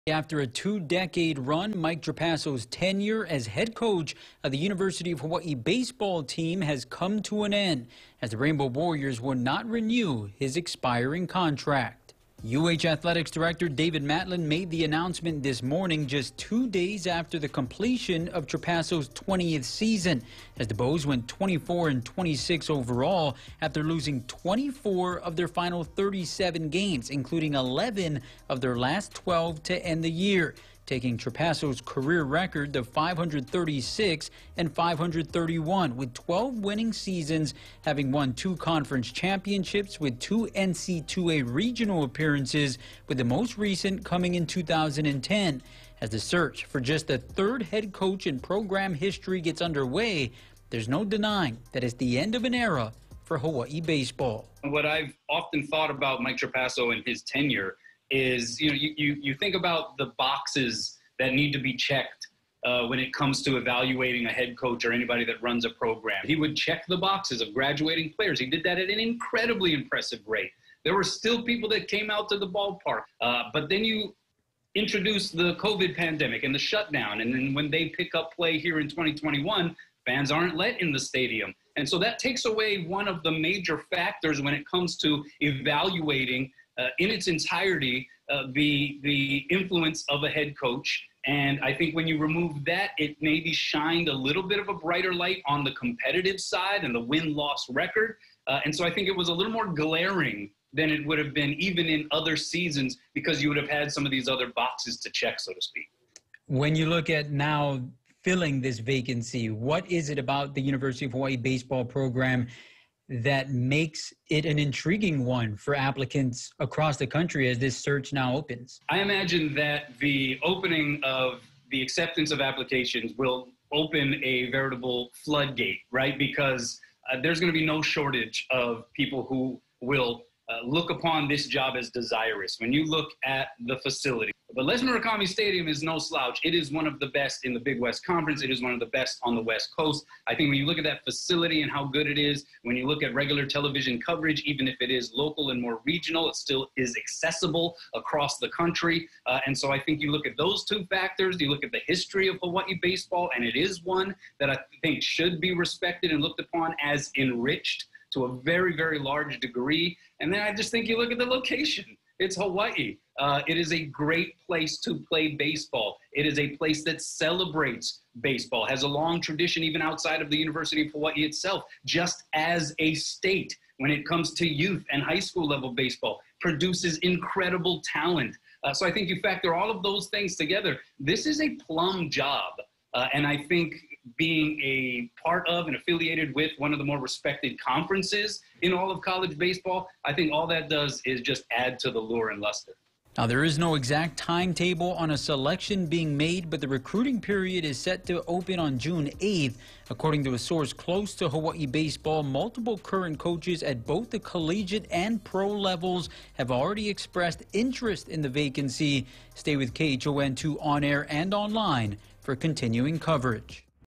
Mike Trapasso out as University of Hawaii baseball coach
Mike Trapasso's tenure as the University of Hawaii baseball coach has come to an end.